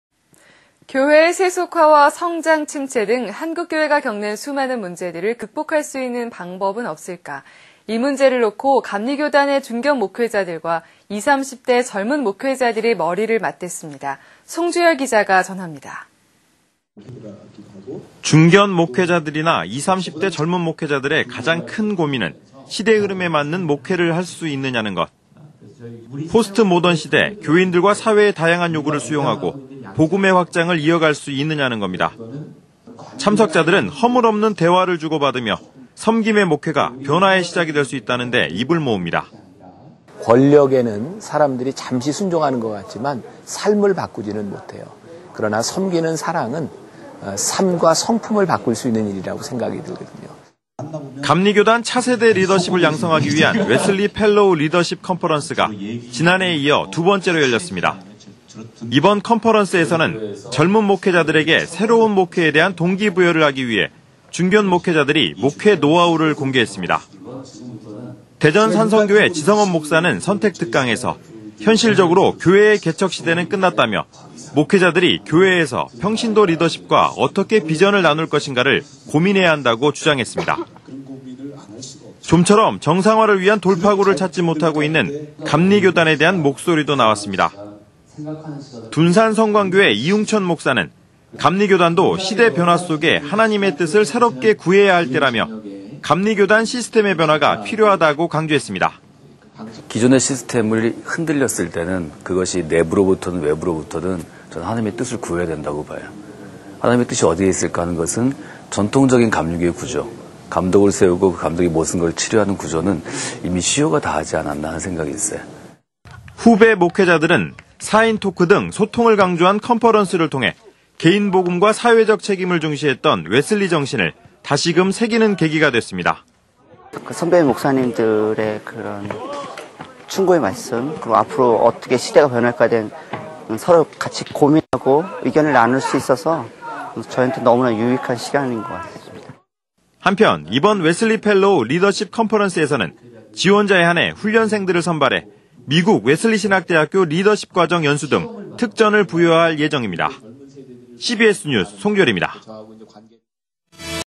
웨슬리 펠로우 컨퍼런스
[ 앵커 멘트 ]

교회의 세속화와 성장 침체 등한국교회가 겪는 수많은 문제들을극복할 수 있는 방법은 없을까?

이 문제를 놓고 감리교단의 중견 목회자들과20,30대 젊은 목회자들이 머리를 맞댔습니다.

송주열 기자의 보도입니다

중견목회자들이나 20,30대 젊은 목회자들의 가장 큰 고민은 시대흐름에 맞는 목회를 할 수 있느냐는 것.

포스트모던시대,
교인들과 사회의 다양한 요구를 수용하고,복음의 확장을 이어갈 수 있느냐는 겁니다.

참석자들은 허물없는 대화를 주고받으며, 섬김의 목회가 변화의 시작이 될 수 있다는 데 입을 모읍니다.

김병삼 목사 / 만나교회
"권력은 사람들이 잠시 순종하는 것 같지만, 삶을 바꾸지 못해요. 섬김의 사랑은 삶과 성품을 바꿀 수 있다.교회가 가장 필요한 것은 사랑이 필요."

감리교단 차세대 리더십을 양성하기 위한웨슬리 펠로우 리더십 컨퍼런스가 지난해에 이어 두 번째로 열렸습니다.

이번 컨퍼런스에서는 젊은 목회자들에게새로운 목회에 대한 동기부여를 하기 위해 중견목회자들이 목회 노하우를 공개했습니다.

대전산성교회 지성업 목사는 선택특강에서"현실적으로 교회 개척의 시대는 끝났다"며,"목회자들이 교회에서 평신도 리더십과 어떻게 비전을 나눌 것인가를 고민해야 한다"고 주장했습니다.

좀처럼 정상화를 위한 돌파구를 찾지 못하고 있는감리교단에 대한 목소리도 나왔습니다.

둔산성광교회 이웅천 목사는
"감리교단도 시대 변화 속에 하나님의 뜻을 새롭게 구해야 할 때"라며, "감리교단 시스템의 변화가 필요하다"고 강조했습니다.

이웅천 목사 / 둔산성광교회
"기존 시스템이 흔들렸을때는 하나님의 뜻을 구해야. 전통적인 구조는 이미 시효가 다하지 않았나. 똑같은 감독제를 유지해도 이전의 영향력과 시스템보다는 새로운 시대에 맞는 시스템 생각"

후배 목회자들은 4인 토크 등 소통을 강조한 컨퍼런스를 통해개인복음과 사회적책임을 중시했던 웨슬리정신을다시금 새기는 계기가 됐습니다.

최태춘 전도사 / 포도나무교회
"교회가 시대요구에 부응하는 것들이 많은데,웨슬리 펠로우에 와서 선배 목사들의 충고의 말씀어떻게 시대가 변화할 것인가에 대한 고민을 나눠서 유익한 시간."

한편, 이번 웨슬리 펠로우 리더십 컨퍼런스에서는지원자에 한해 웨슬리 펠로우 훈련생들을 선발해미국 웨슬리신학대학교 리더십 과정 등 특전을 부여할 예정입니다.

CBS뉴스 송주열입니다.